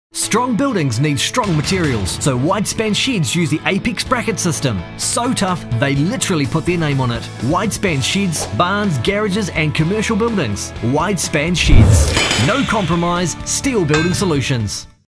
Wide Span Sheds NZ - TV Commercial
Check out our Television Commercial which was made up by Wide Span Sheds in Ashburton.